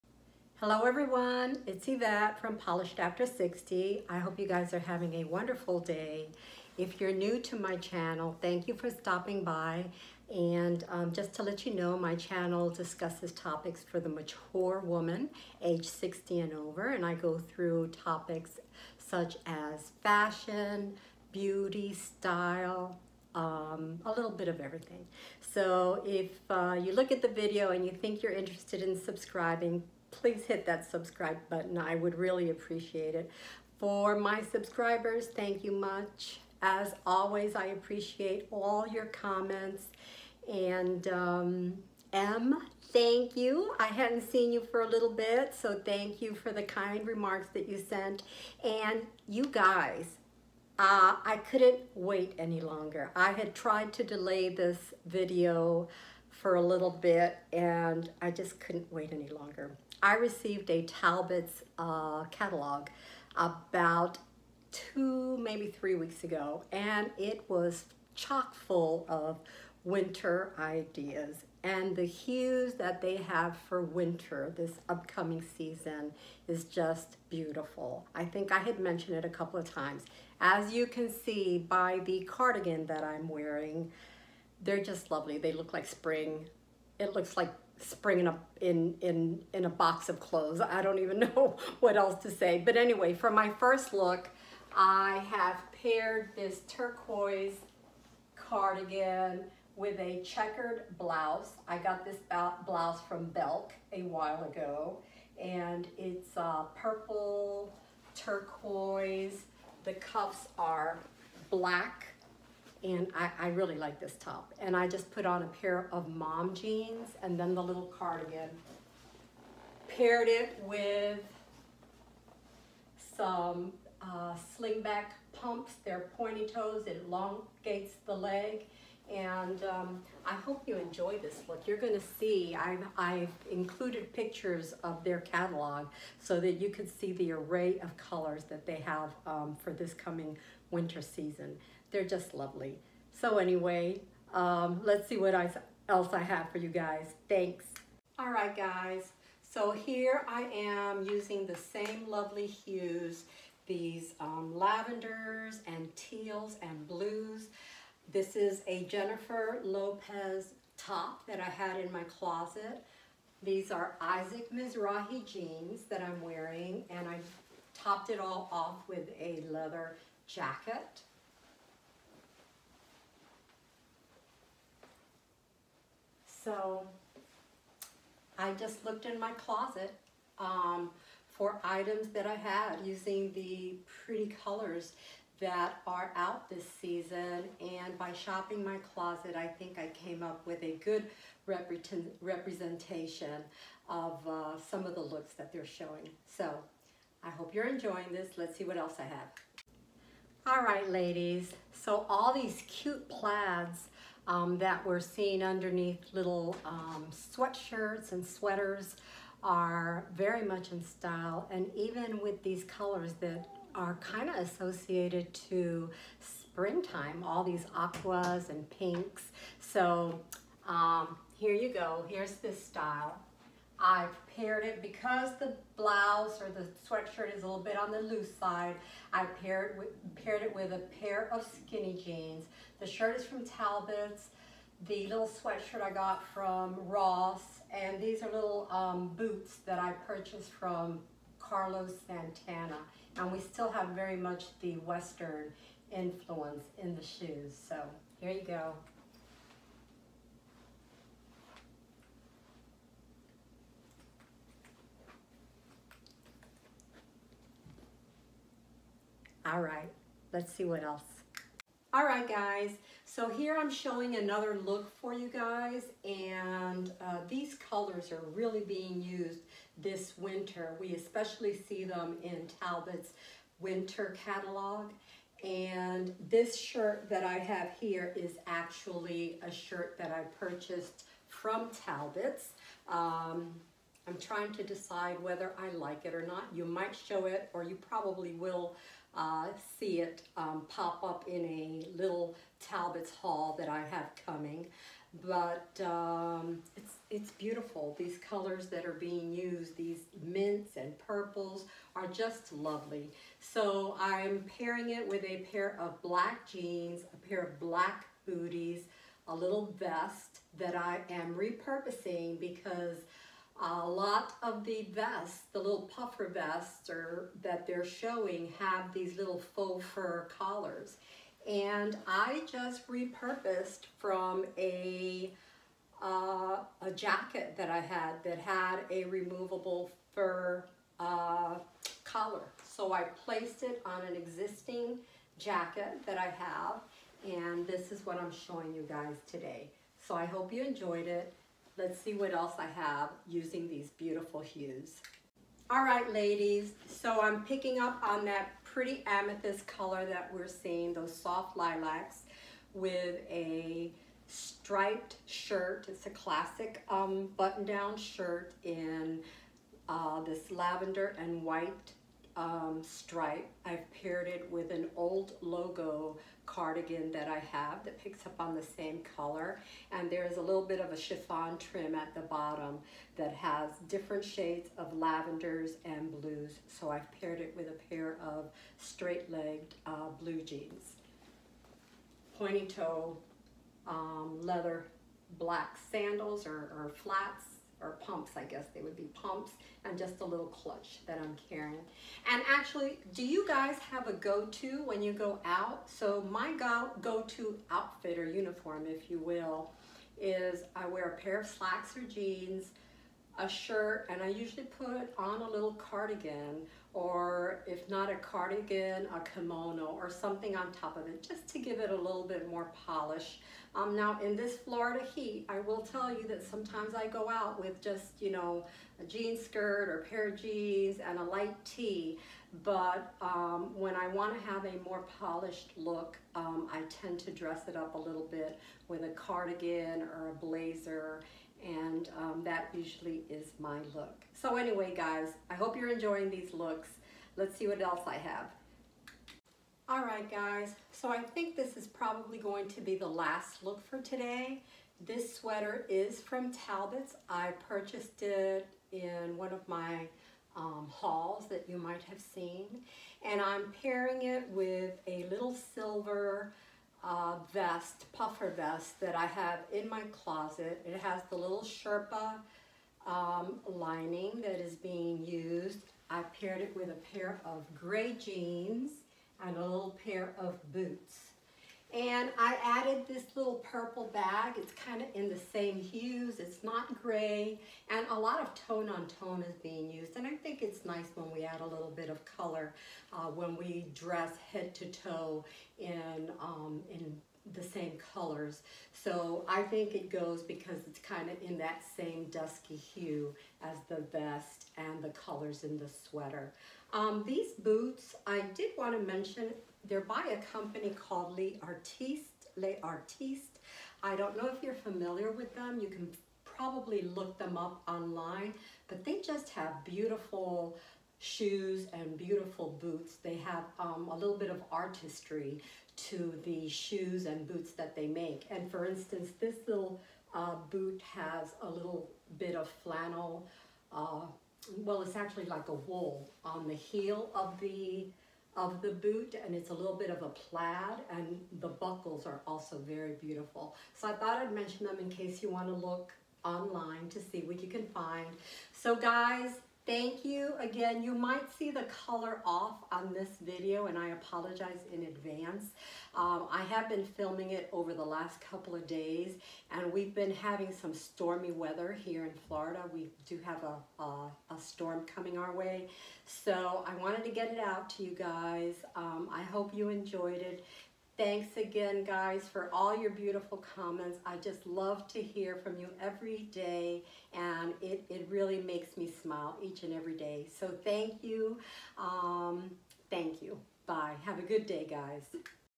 Winter Hues Talbots Lookbook Fashion over 60
Hello everyone, I hope you’re having a wonderful day. Today I am showing a new shirt that I just received from Talbots in some of their winter hues. I am also showcasing a sweater from a past haul, as well as, a blouse.
The winter catalog is full of beautiful colors; shades of blue, lavender, teals. In addition the typical shades of grey and black.
Please give me your idea on the new shirt that I just received. I don’t think it’s as flattering as other items I’ve purchased from them. I’m leaning toward returning it. So sad as I loved the colors.
Also I would love to hear what your standard “uniform” is. Are you most comfortable in dresses and skirts? Or like me do you have a look of slacks, shirt, and blazer, cardigan or kimono.
I love all style as you know if you’ve been following my videos; but I do have a go to look that I can always count on.
Kindest Regards

Yvette
Polished After Sixty